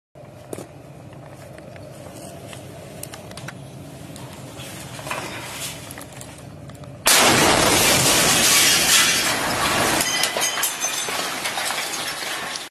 Russian T80 fired at a cameramen , fire to residential building
people are speculating the RI signature from the camera is what made tank engage